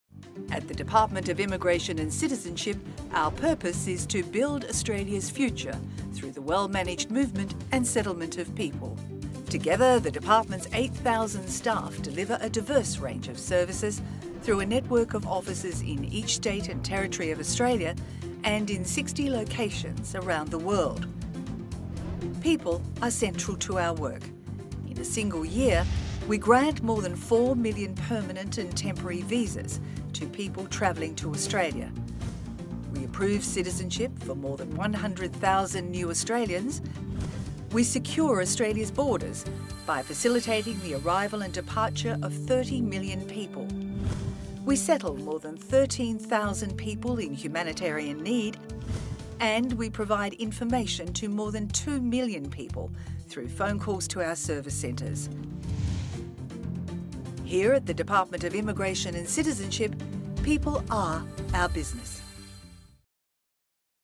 A peek into DIAC
The Department of Immigration and Citizenship plays a key role building Australia's future through the well-managed movement and settlement of people.

People are our business.